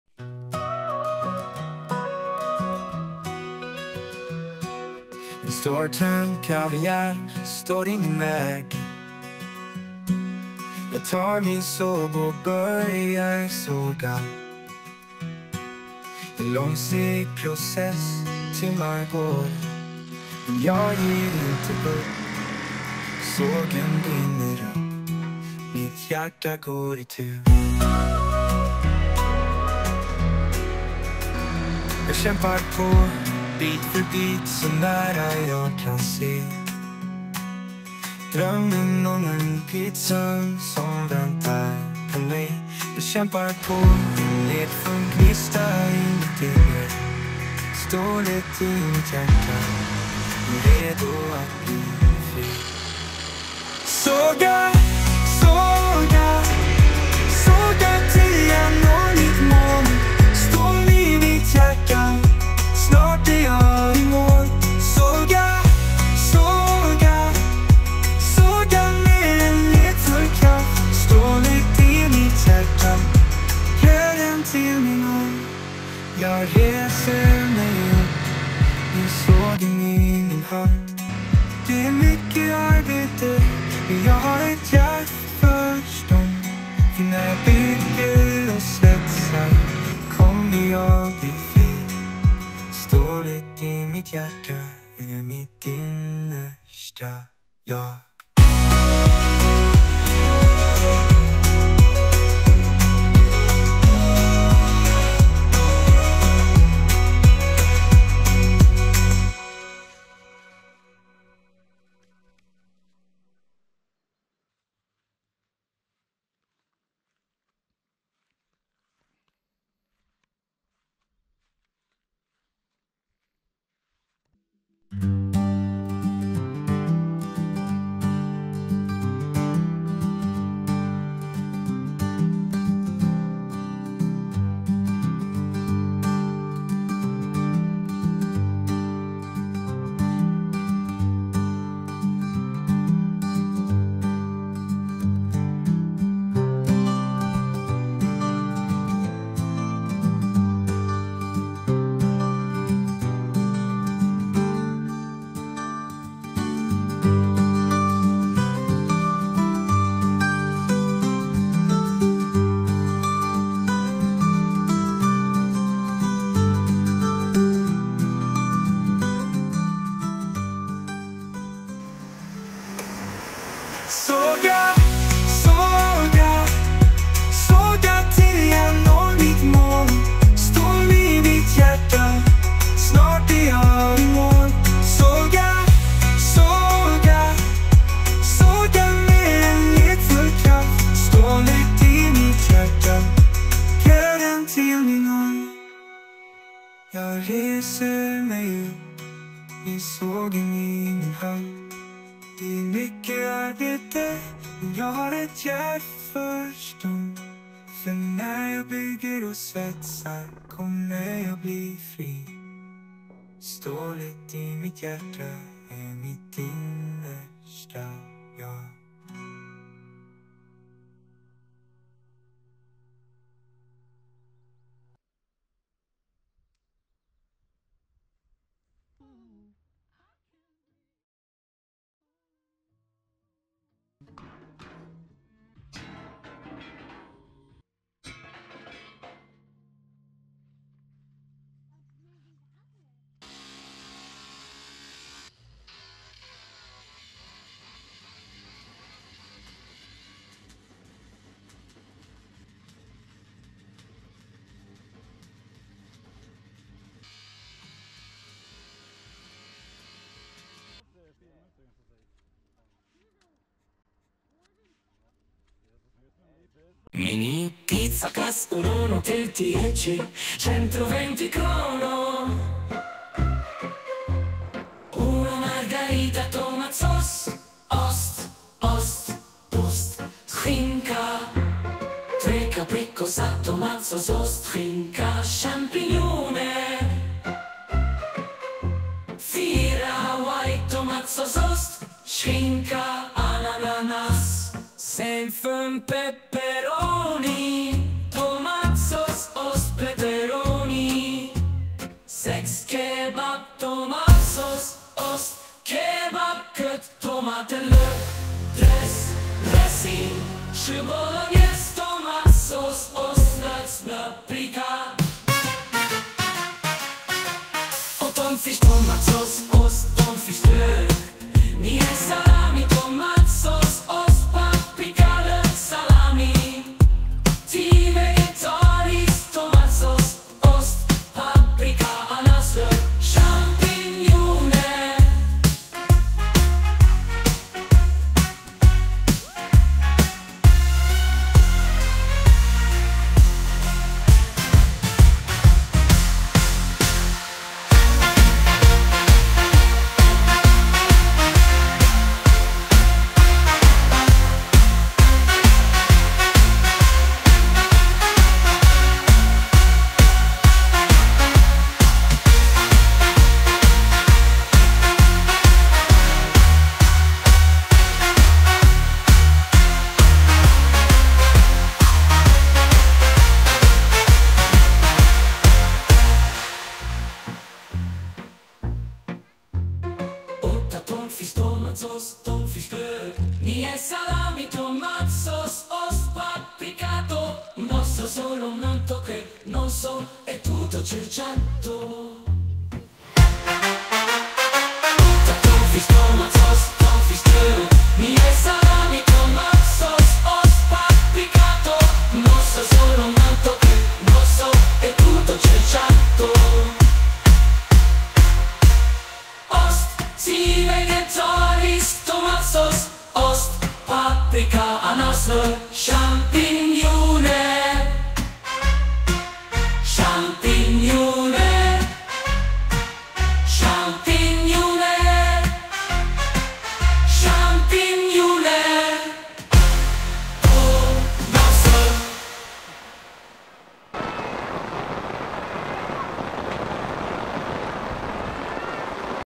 Building a pizza oven and having a pizza party in the rain
Building, sawing, grinding and welding togeher a pizza oven from an old iron tank and then inaugurating it with a pizza party.

The music is made by us and suno.  Songs can be found at @sandboxjunkyardaimusic